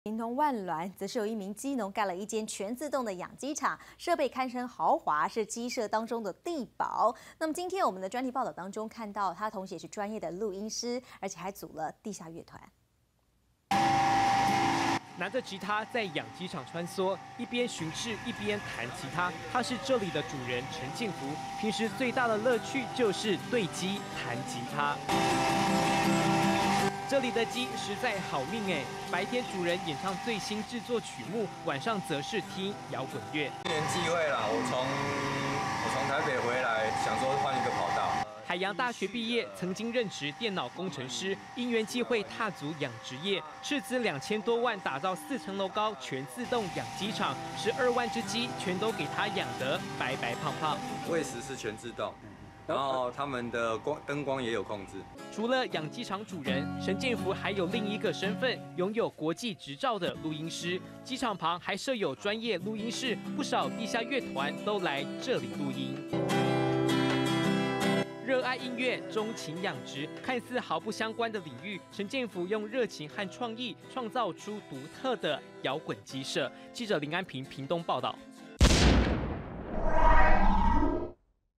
壹電視新聞》雞舍版帝寶！全自動養殖 夜播搖滾樂
【壹電視報導】
屏東萬巒有一位雞農蓋了一間全自動的養雞場，設備豪華，堪稱是雞舍中的帝寶，同時他也是專業的錄音師，還組有地下樂團，平時創作歌曲，就會先到廠房用吉他彈給雞聽，就連晚上都放搖滾樂，朋友們都稱呼他雞舍裡的搖滾王子。

敢說真話的壹電視新聞台
．中華電信MOD 56 頻道
．全台有線電視 45頻道